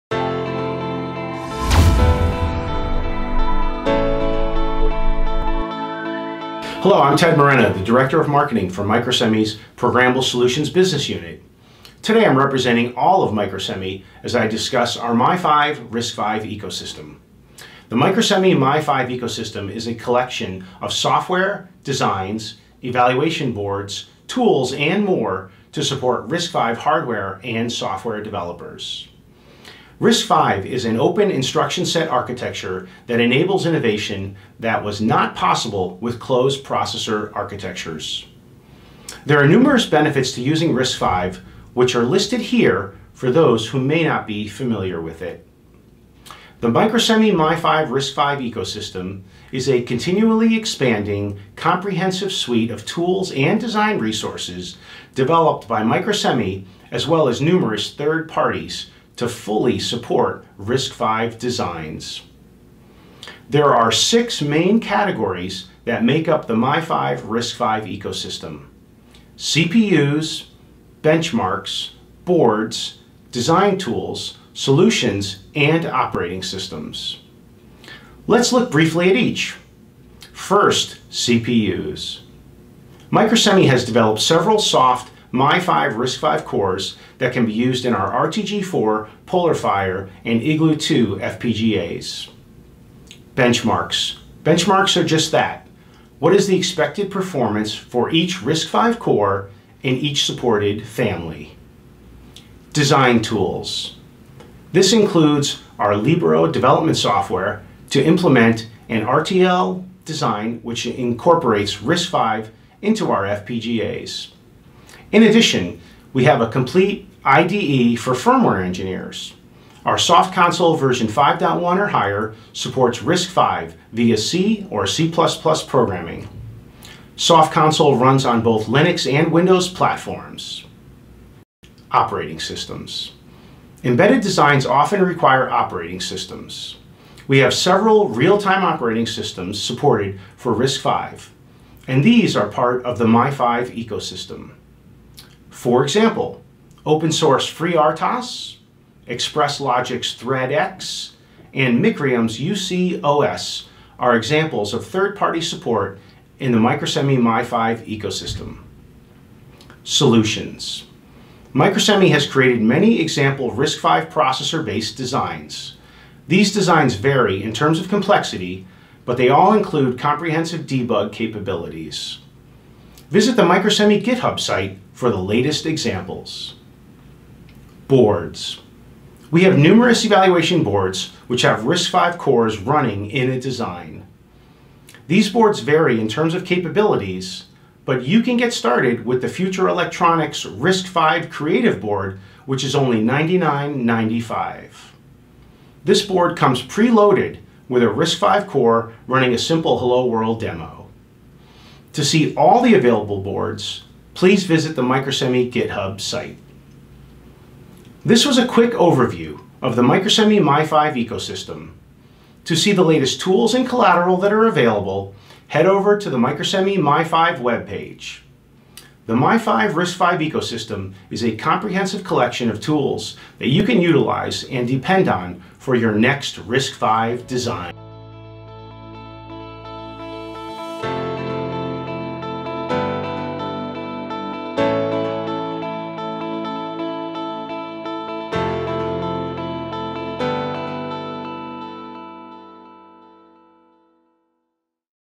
Microsemi Mi-V Ecosystem for RISC-V Developers
Microsemi’s Mi-V ecosystem supports RISC-V hardware and software developers with the latest software, designs, development boards and tools.